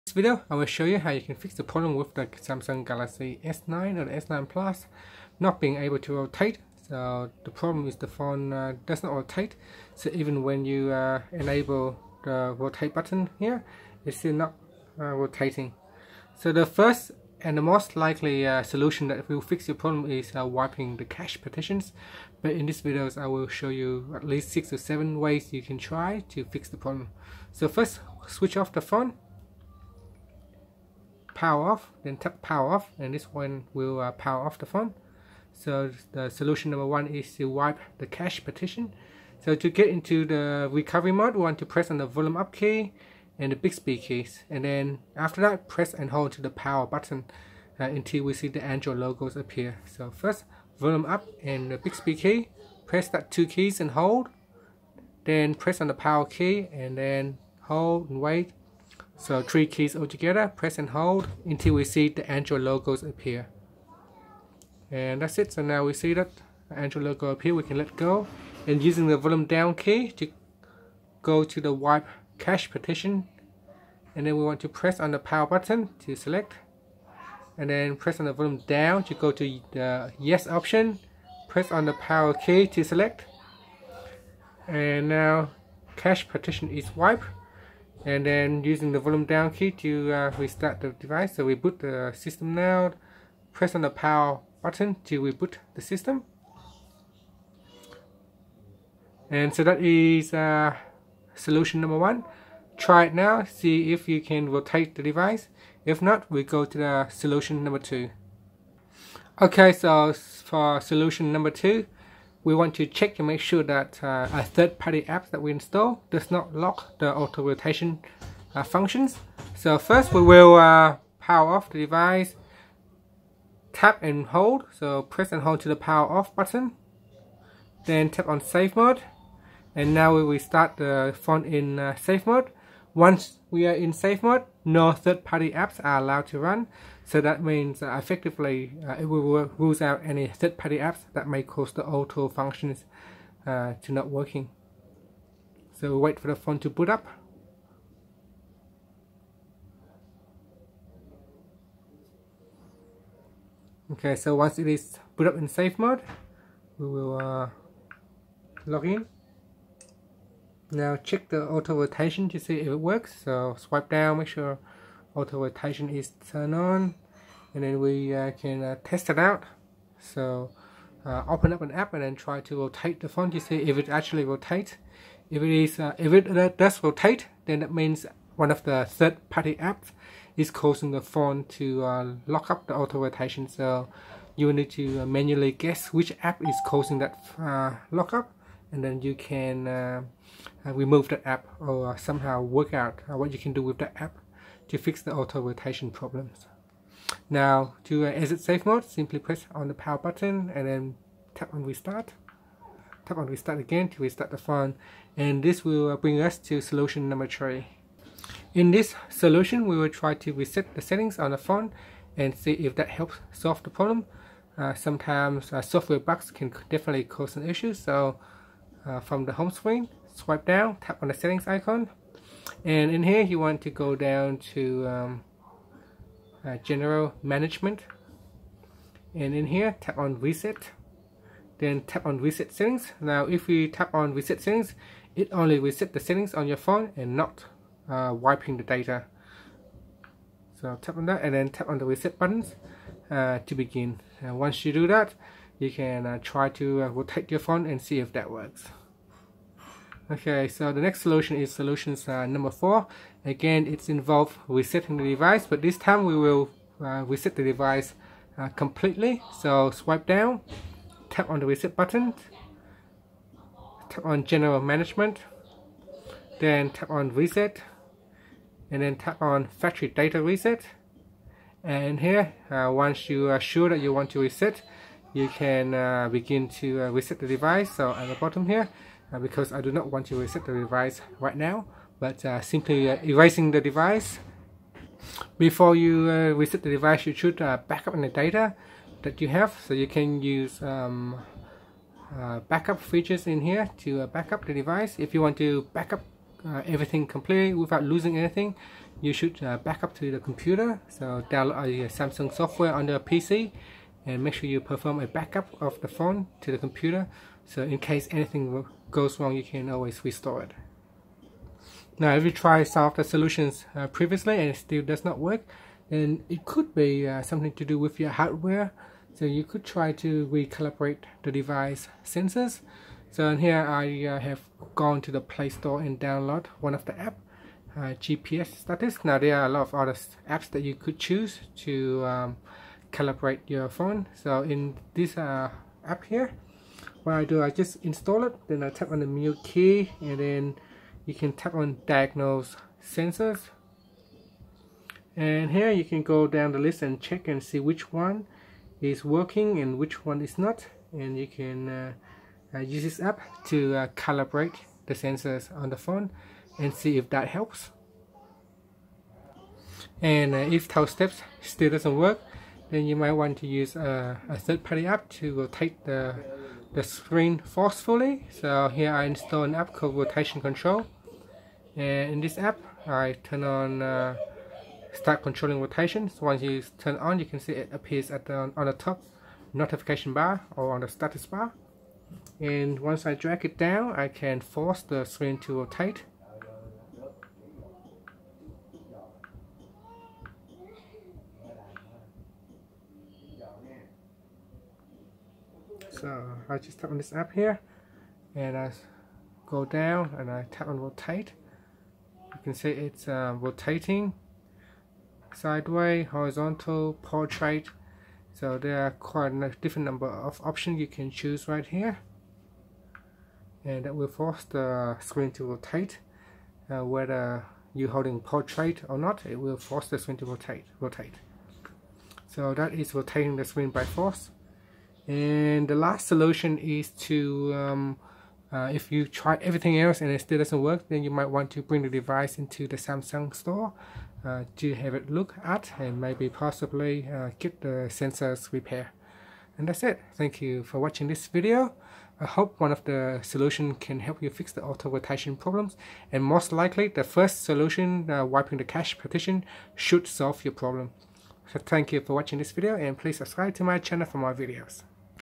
Samsung Galaxy S9 / S9+: 7 Solutions to Fix Stuck Auto Rotation
See the seven different solutions you could try to fix the stuck auto rotation on the Samsung Galaxy S9 / S9+.